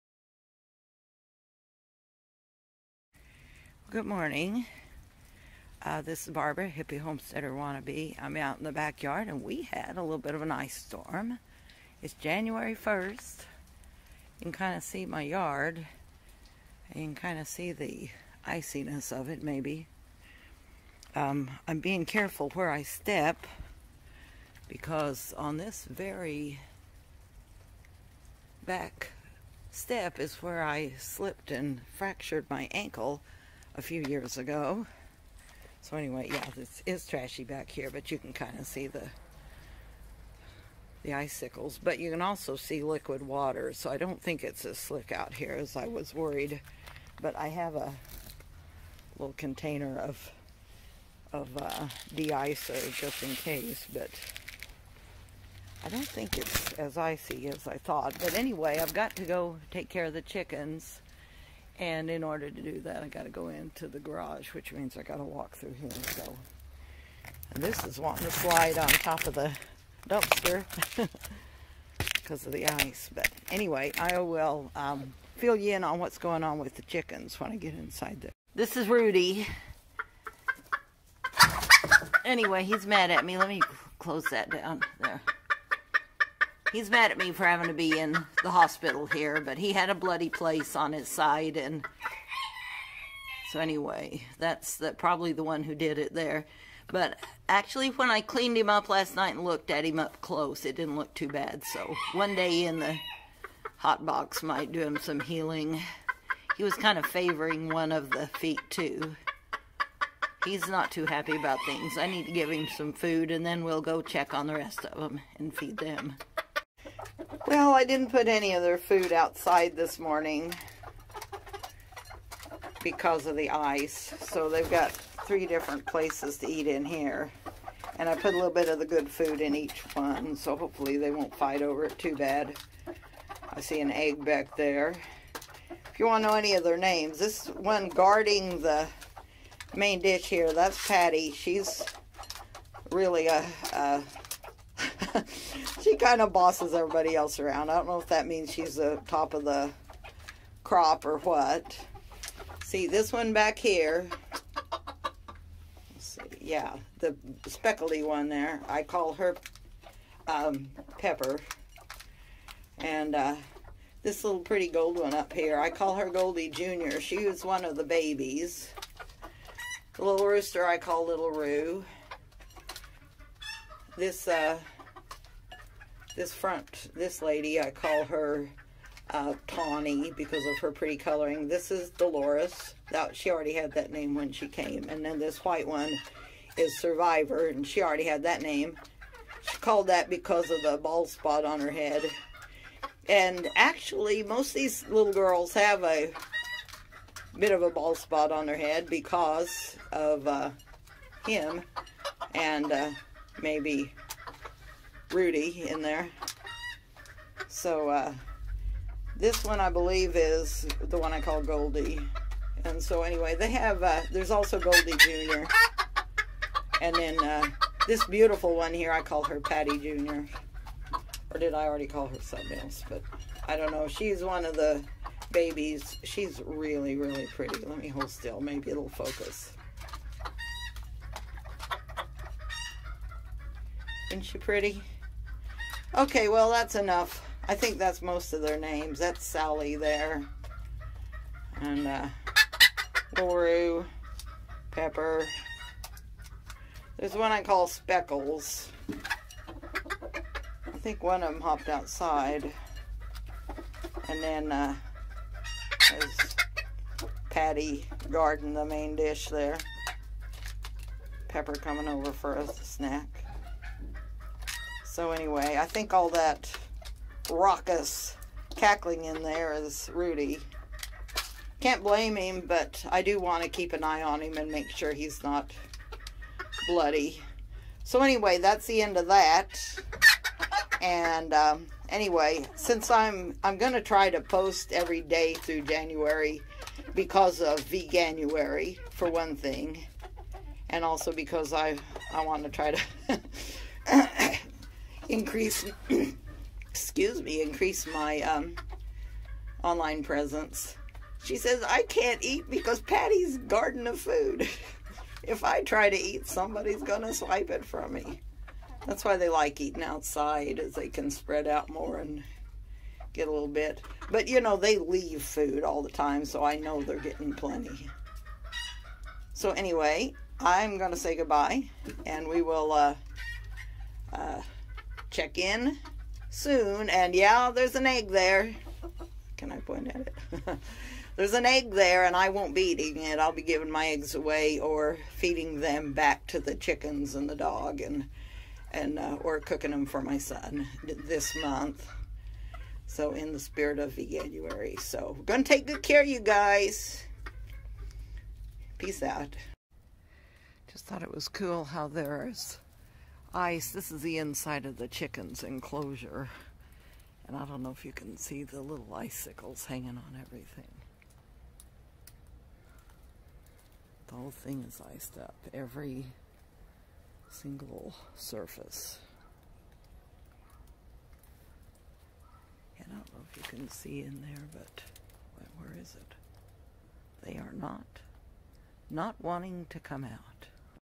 Rooster Injury, Ice Storm, and Veganuary Day 2
Not a lot about vegan eating here, but you can see the chickens and learn some of their names.